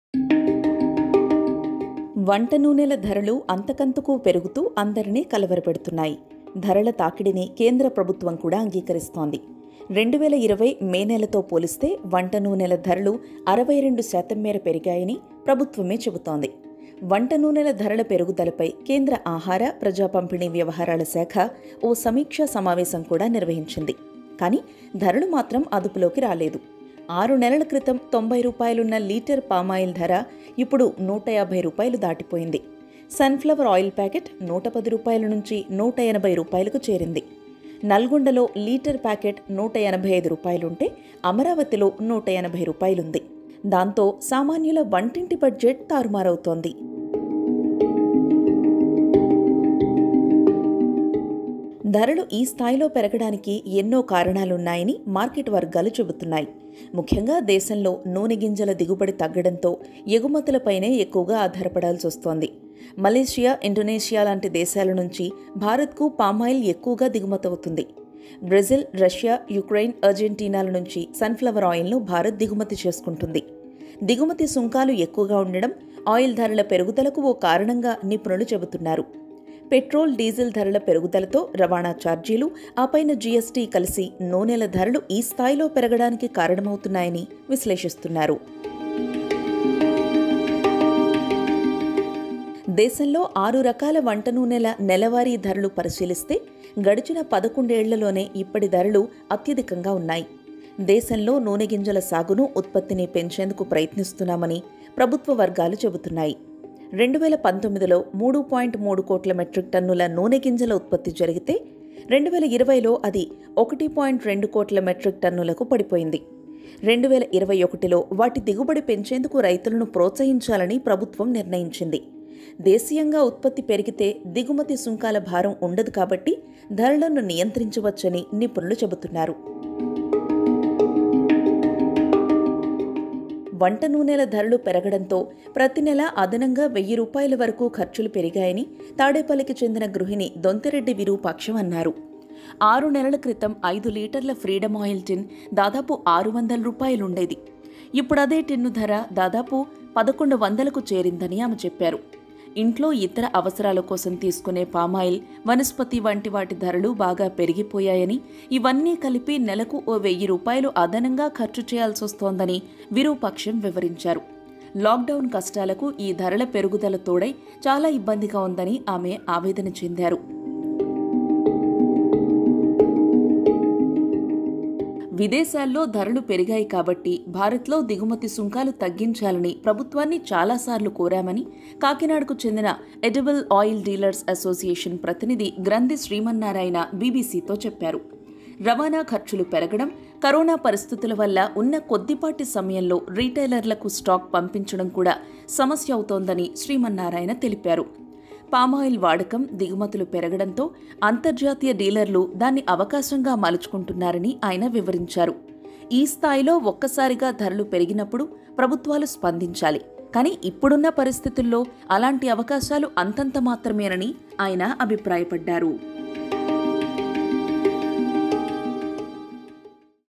Cooking Oil: 11 ఏళ్లలో ఎప్పుడూ లేనంతగా వంటనూనెల ధరలు.. తగ్గాలంటే ప్రభుత్వాలు ఏం చేయాలి? | BBC Telugu
వంటనూనెల ధరలు గత 11 ఏళ్లలో ఎప్పుడూ లేనంత అత్యధిక స్థాయికి చేరుకొన్నాయి. లాక్‌డౌన్ కష్టాలకు ధరల పెరుగుదల తోడై సామాన్యులను ఇంకా ఇబ్బంది పెడుతోంది. ఆరు నెలల క్రితం 90 రూపాయలున్న లీటర్ పామాయిల్ ధర ఇప్పుడు 150 రూపాయలు దాటిపోయింది. సన్‌ఫ్లవర్ ఆయిల్ ప్యాకెట్‌ 110 రూపాయల నుంచి 180 రూపాయలకు చేరింది. నల్గొండలో లీటర్ ప్యాకెట్ 185 రూపాయలుంటే, అమరావతిలో 180 రూపాయలుంది.

(వీడియోలో 1:00  వద్ద 'దిగుమతుల' బదులు 'ఎగుమతుల' అని పొరపాటున వచ్చింది. గమనించగలరు.)